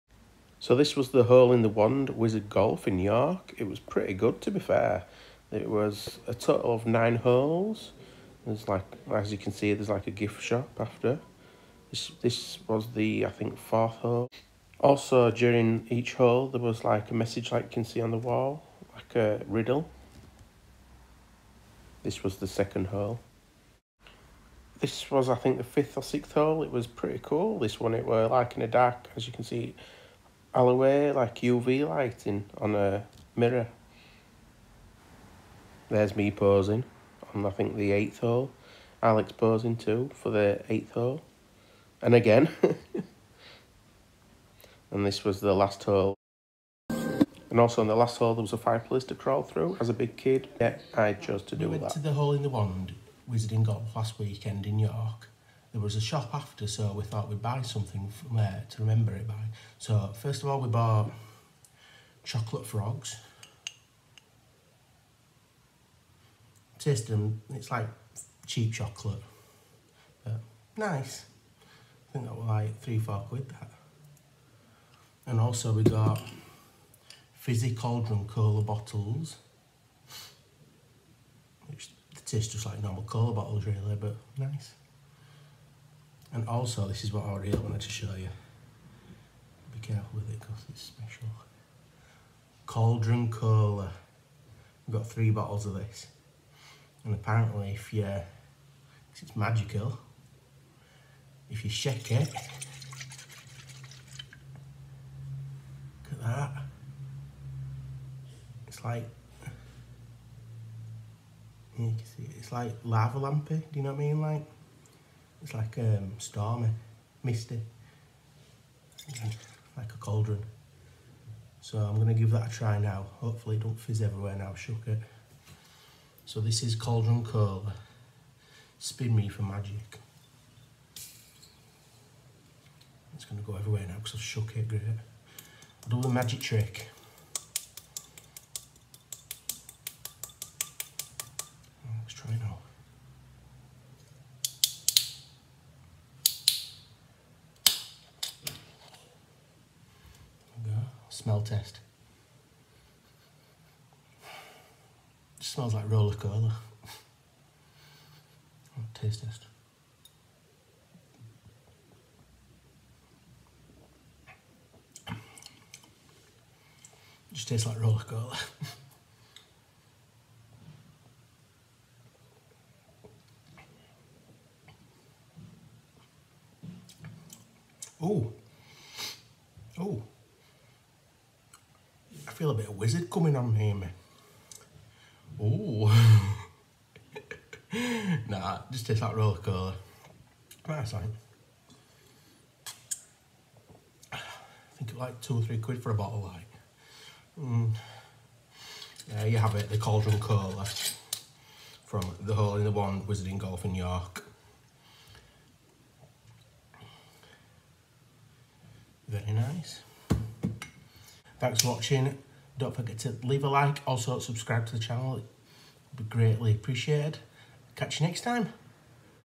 WE WENT TO THE HOLE IN WAND WIZARD GOLF IN YORK! 🧙🏼‍♀️
So we decided to try out the Hooe in wand wizard golf which is based in York. it’s worth it kids will love it, it’s for all ages really.

So enjoy